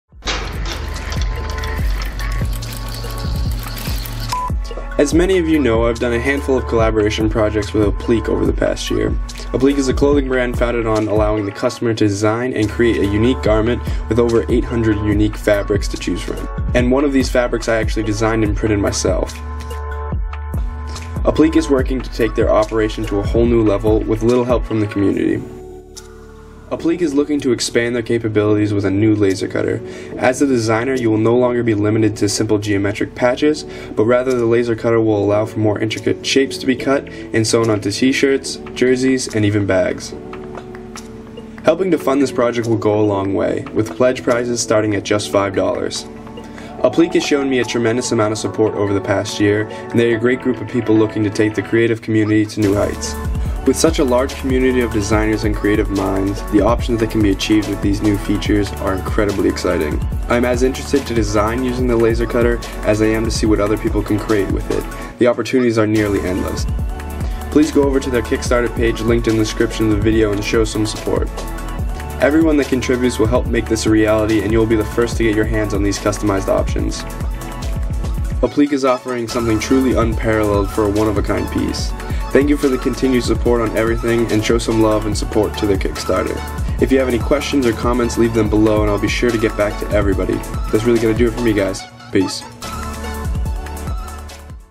Designing Custom Clothing Without Limits
Apliiq has shown me a tremendous amount of support and it's time to return the favor. Head over to their Kick Starter page and pledge toward their project. Some really great options on any budget. And even if you can't support it directly spread the word in any way you see fit.

KL / Plndr Rep Code SIVE